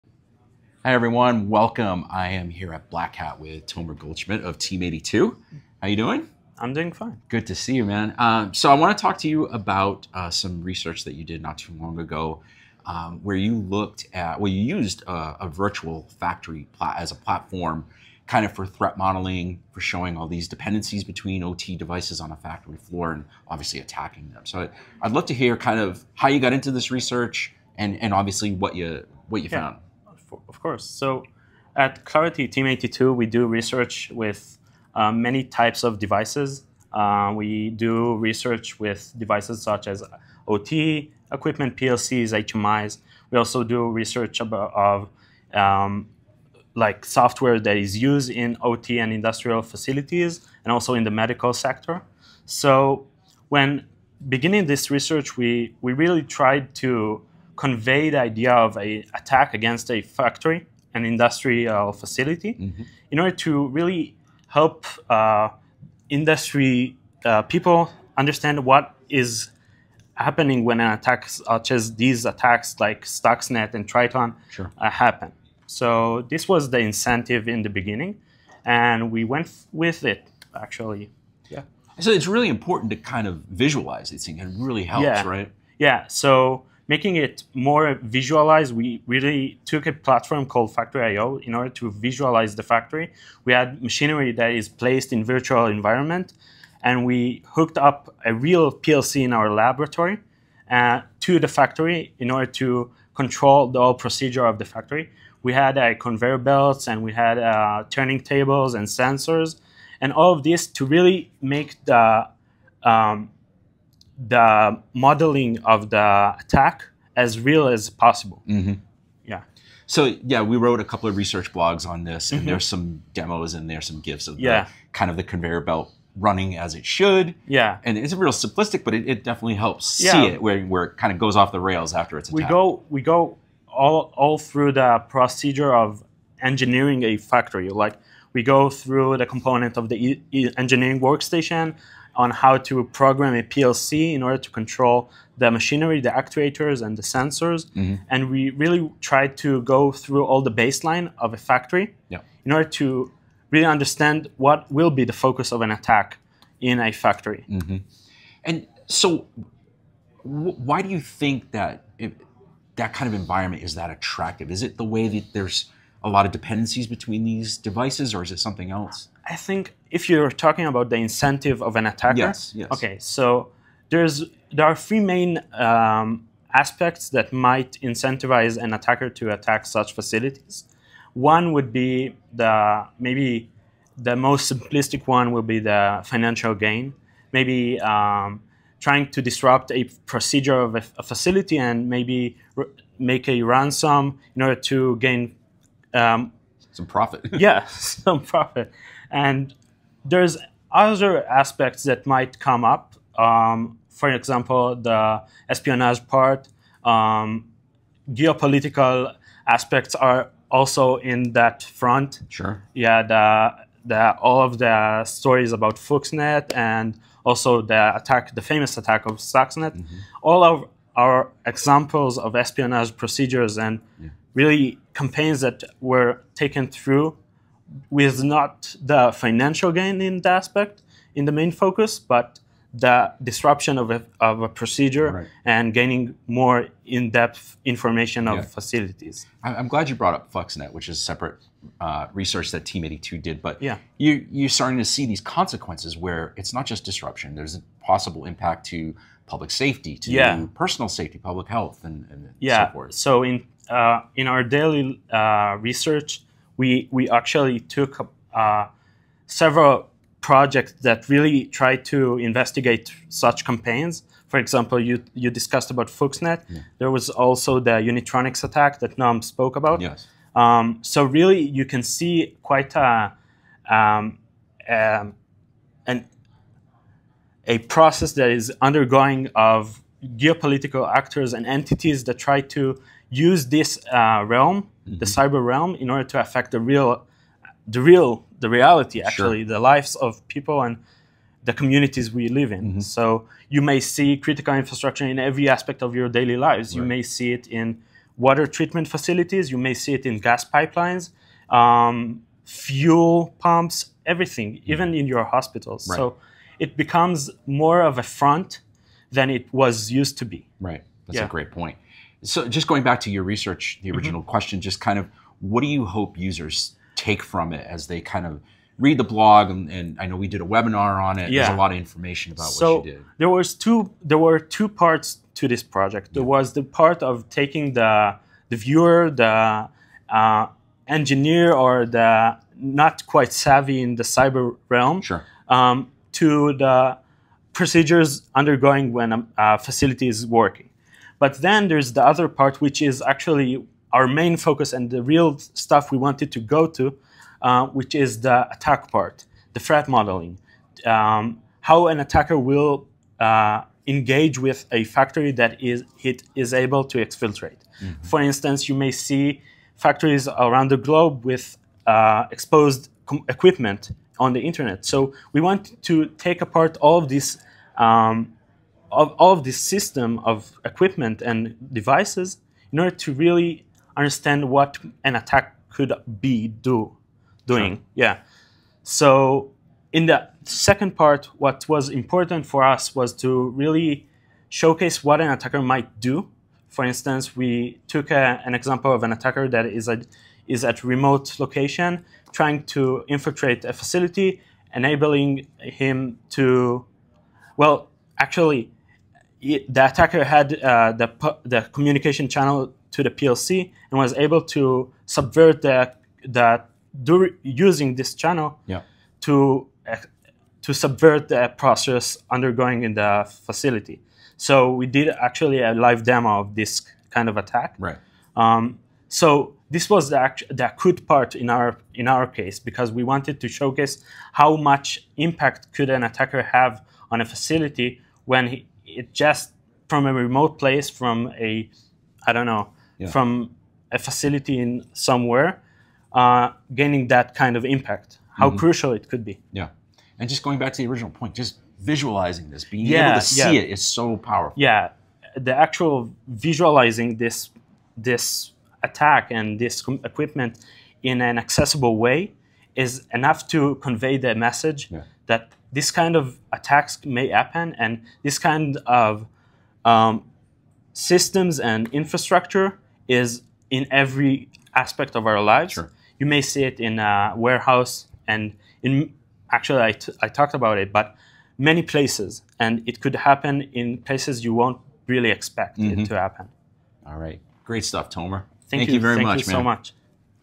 Claroty at Black Hat USA 2024 - Tomer Goldschmidt
Claroty Vulnerability Researcher, Tomer Goldschmidt, joins Mike Mimoso at The Foundation Room during Black Hat USA 2024 to talk about Team82's threat modeling for industrial environments using a virtual factory called Factory/IO. Read the blog